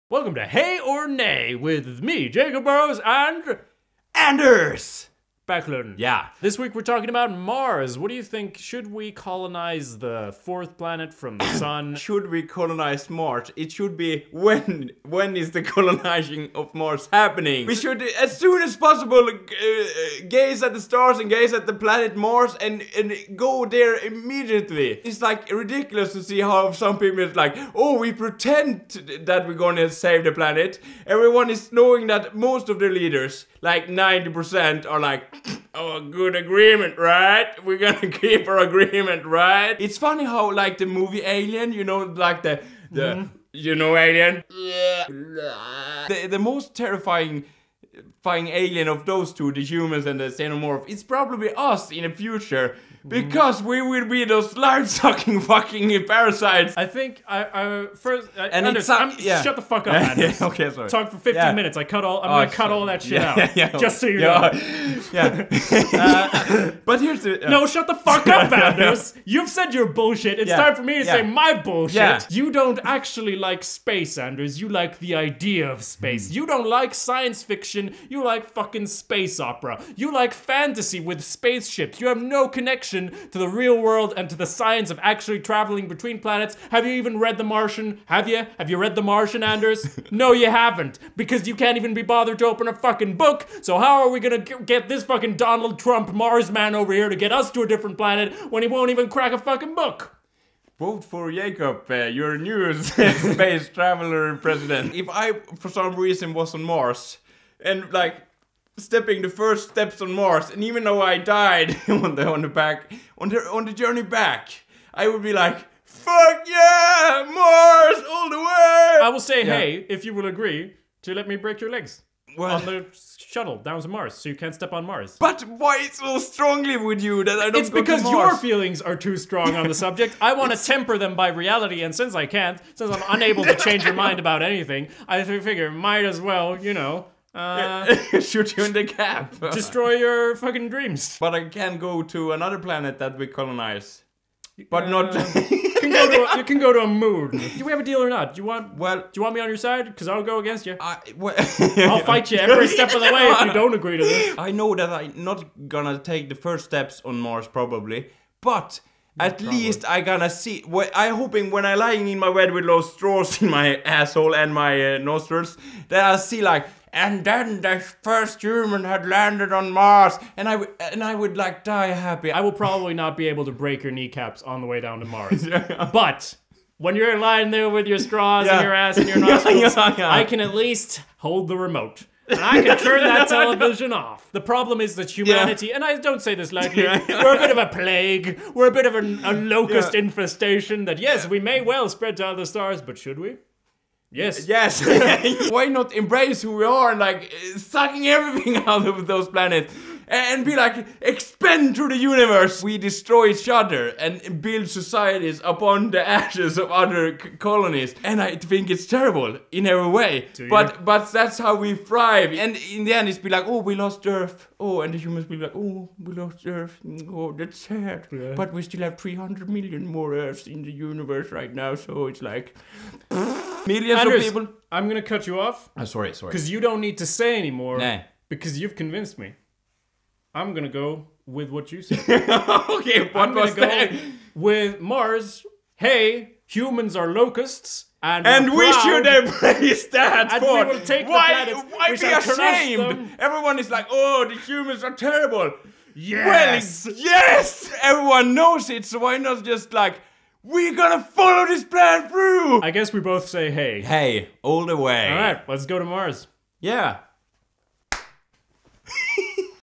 Should Humanity Colonize Mars?
Should humanity colonize Mars or try to fix Earth? Could we live on Mars? WHY should we go to Mars? We solve this complicated issue by yelling at each other, as usual.

Shot this video with a Panasonic GH4 and a 14-42mm f/3.5–5.6 lens.

Remember to share this, and to tell everyone to chill out about this whole Mars thing because we solved it already and we are all locusts yaaaay